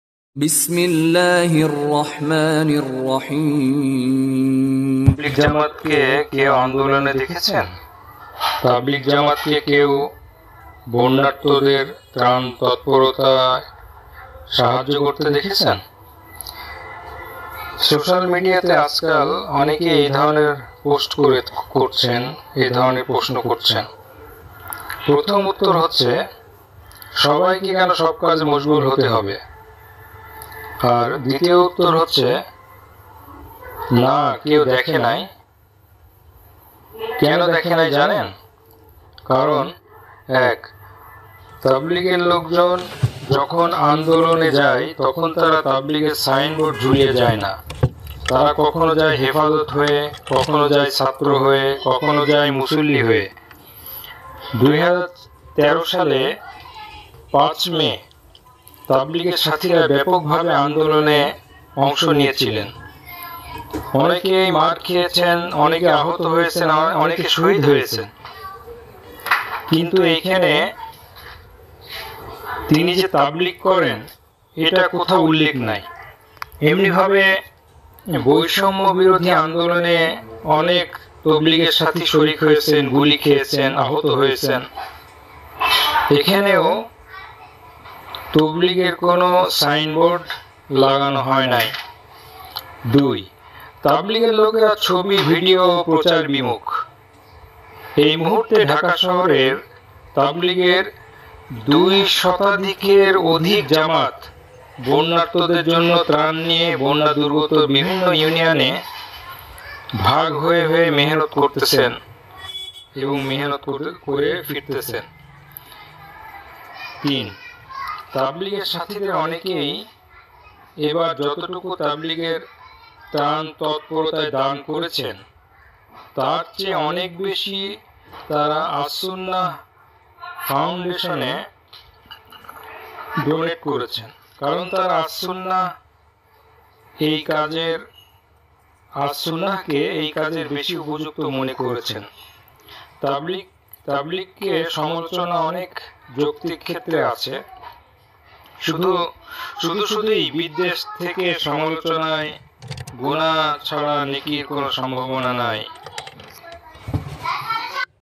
তাবলীগ জামাত কে কেন আন্দোলনে দেখা যায় না || tabligi jamat the movement
তাবলিগ জামাতকে কেউ আন্দোলনে দেখেছেন?
তাবলিগ জামাতকে কেউ বন্যার্তদের ত্রাণ তৎপরতায় দেখেছেন?
অনেকেই এ ধরণের পোস্ট করছেন।

প্রথম উত্তর হচ্ছেঃ
সবাইকে কেন সব কাজে মশগুল হতে হবে? (আমার আগের পোস্টটা দেখে নেবেন)

দ্বিতীয় উত্তর হচ্ছেঃ
না কেউ দেখে নাই। কেন দেখে নাই? কারণ,

১. তাবলিগের লোকজন যখন আন্দোলনে যায় তখন তারা তাবলিগের সাইনবোর্ড ঝুলিয়ে যায় না, তারা কখনো যায় হেফাজত হয়ে, কখনো ছাত্র  হয়ে, কখনো মুসল্লি হয়। ২০১৩ সালে ৫মে তাবলিগে সাথীরা ব্যাপকভাবে আন্দোলনে অংশ নিয়েছিল, অনেকেই মার খেয়েছে, আহত হয়েছে। বৈষম্যবিরোধী আন্দোলনে শরিক হয়ে অনেকেই গুলি খেয়েছে, আহত হয়েছে।

২. তাবলিগের লোকেরা ছবি, ভিডিও ও প্রচারবিমুখ। এই মুহূর্তে ঢাকা শহরের তাবলিগের দুই শতাধিক জামাত বন্যার্তদের জন্য ত্রাণ নিয়ে বন্যা-দুর্গত বিভিন্ন ইউনিয়নে ভাগ হয়ে মেহনত করছেন বা মেহনত করে ফিরে এসেছেন।

৩. তাবলিগের সাথীদের অনেকেই এবার যতটুকু তাবলিগের ত্রাণ তৎপরতায় দান করেছেন, তার চেয়ে অনেক বেশি আস-সুন্নাহয় ডোনেট করেছেন। কারণ তারা আস-সুন্নাহকে এই কাজের বেশি উপযুক্ত মনে করেছেন।

তাবলিগকে সমালোচনার অনেক যৌক্তিক ক্ষেত্র আছে, শুধু শুধুই বিদ্বেষ থেকে সমালোচনায় গুণাহ ছাড়া নেকির কোন সম্ভাবনা নেই।